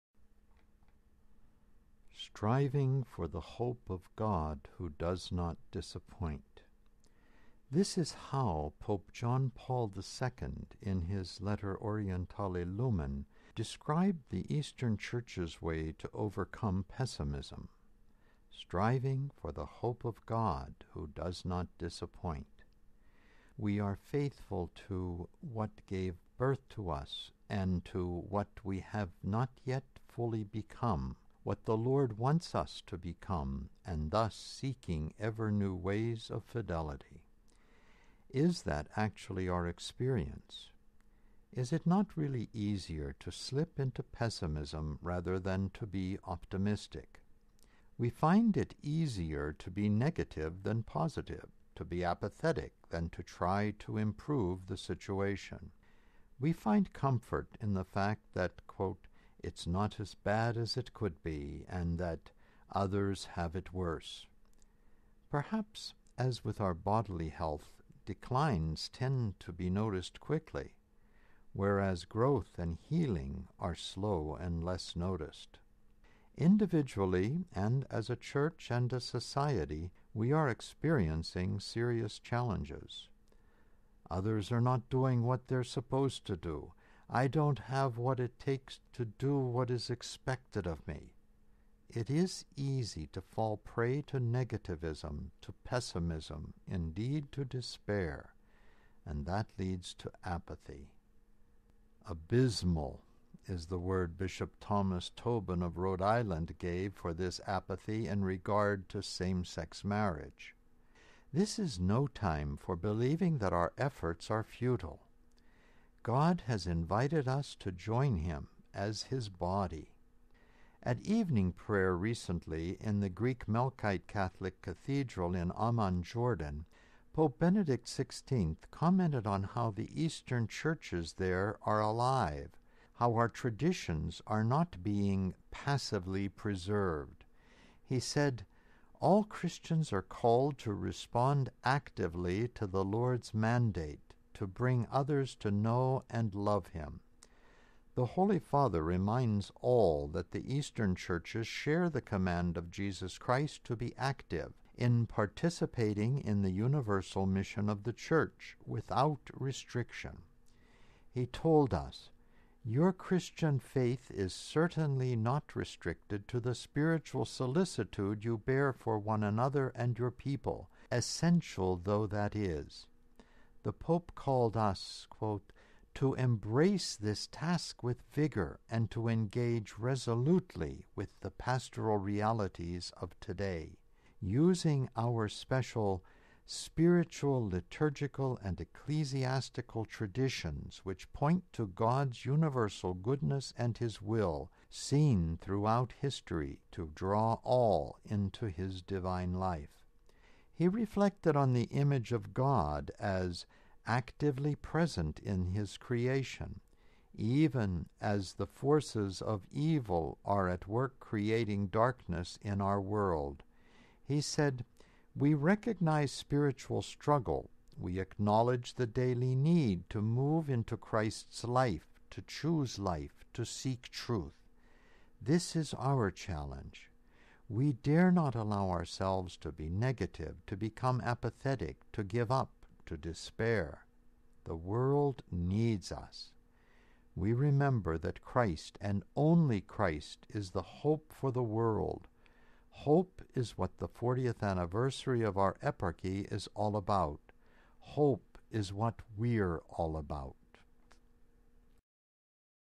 Not negativism, but hope
- Voice of the Shepherd - His Grace, The Most Rev. John Kudrick - Byzantine Catholic Eparchy of Parma, Ohio - May 24, 2009